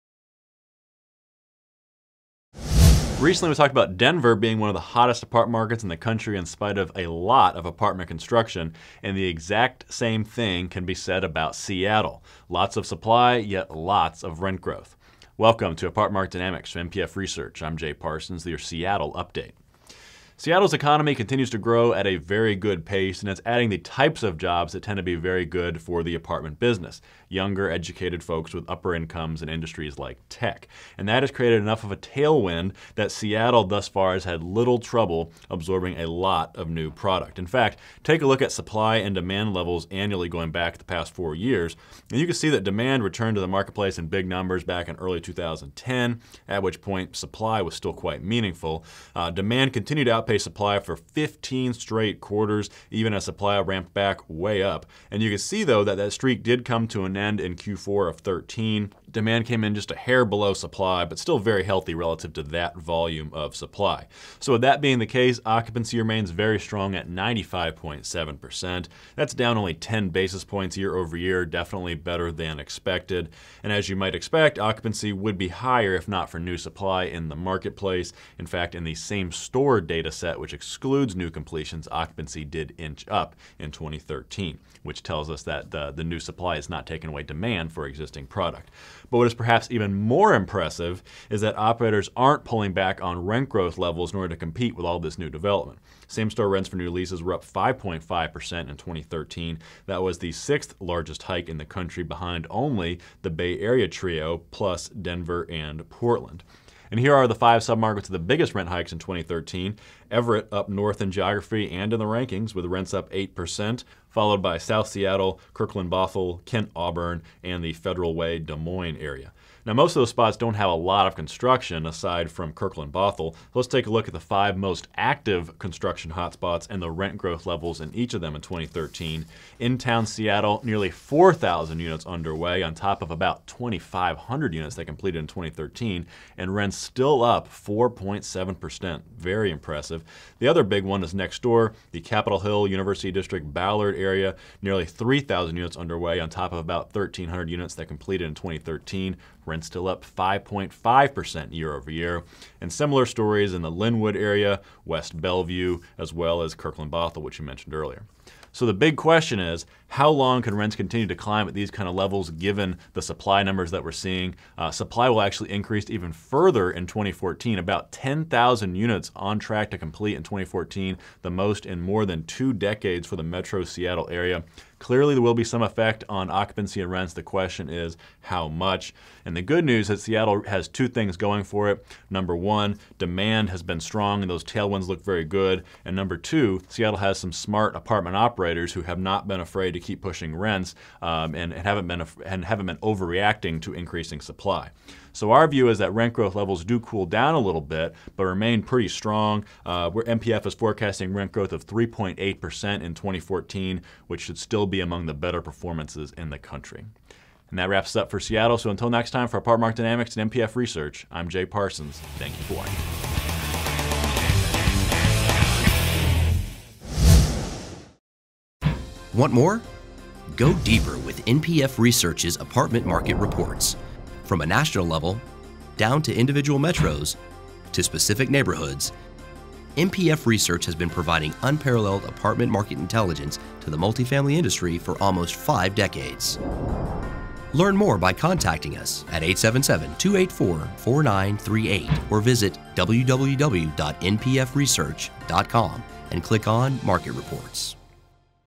Supply Surge Doing Little to Slow Down Rent Growth in Seattle - Apartment Market Dynamics
New apartment supply levels are surging to long-term highs in Seattle, yet so far, that spike hasn't had much effect on apartment fundamentals. Occupancy rates remain strong, and rent growth levels robust.